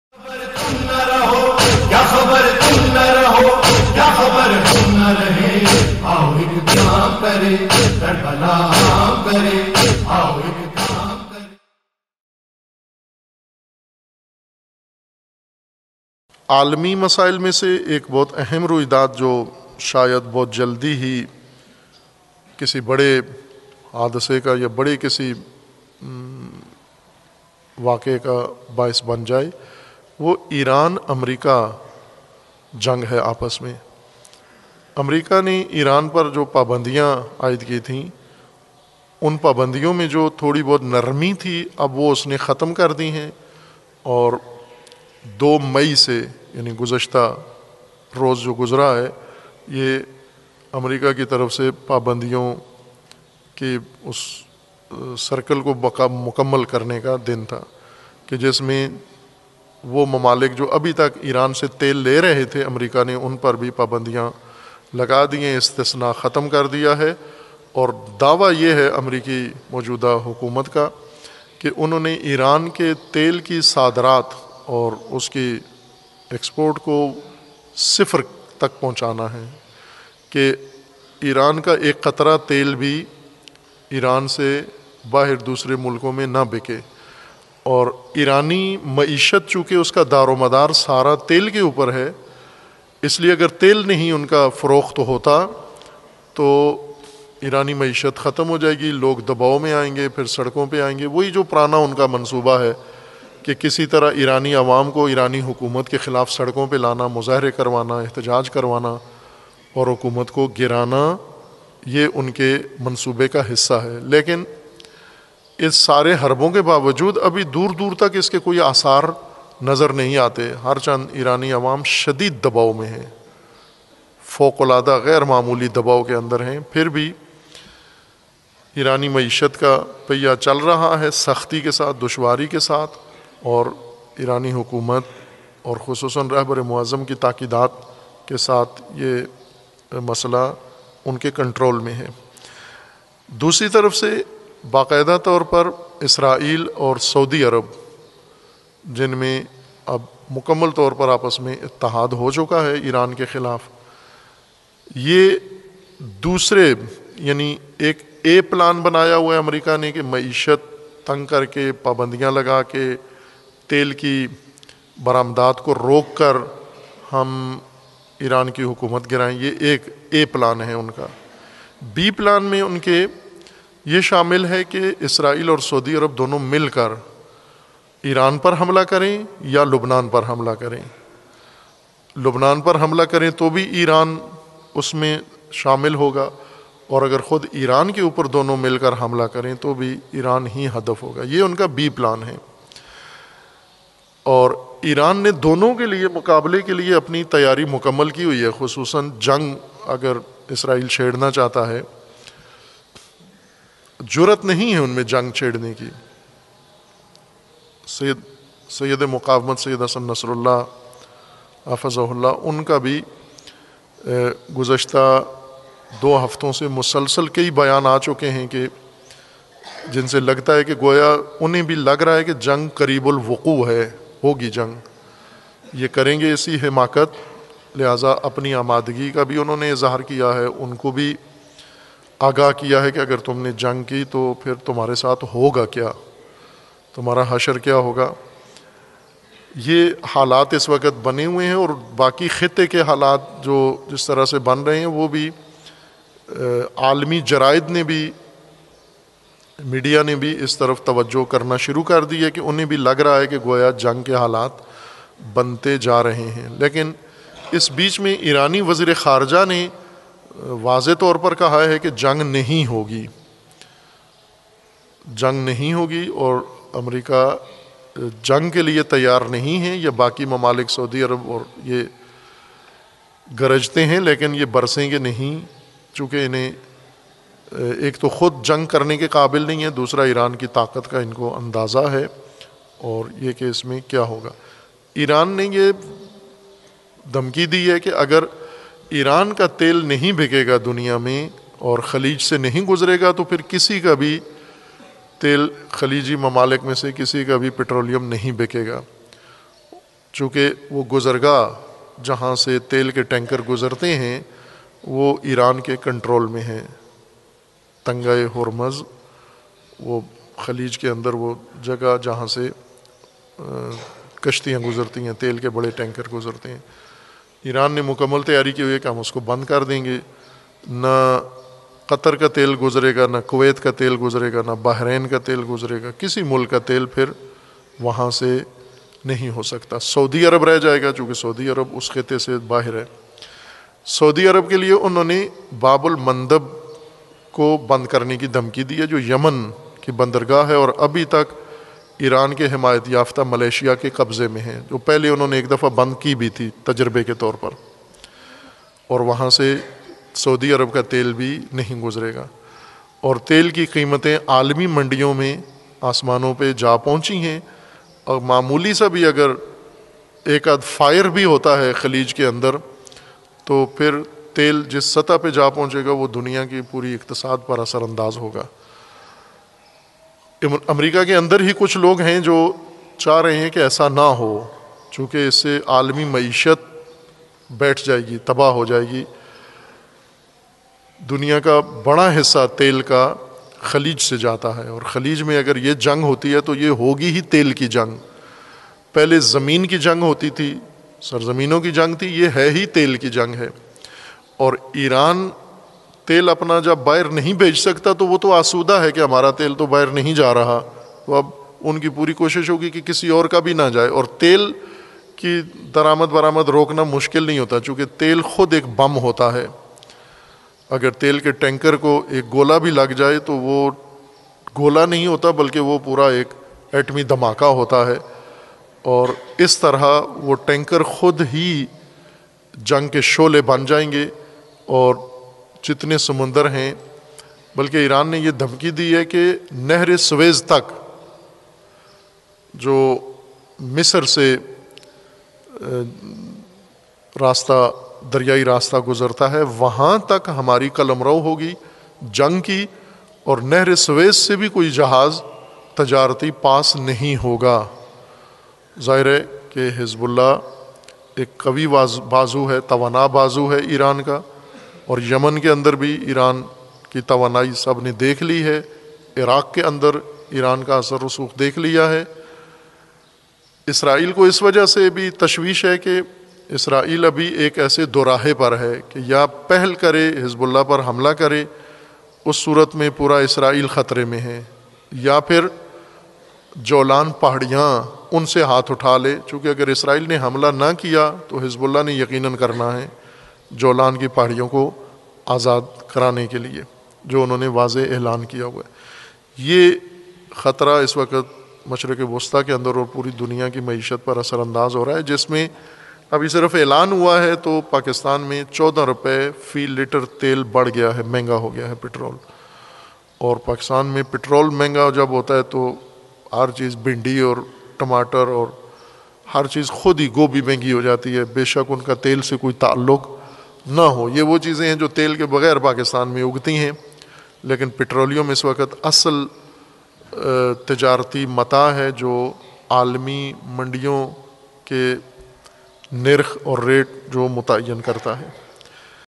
America ka Iran Oil Baraamad Per Pabandi Lugana || Allama Jawad Naqvi
Topic : America ka Iran Oil Baraamad Per Pabandi Lugana
Main Highlight :America,Iran
Speaker : Allama Jawad Naqvi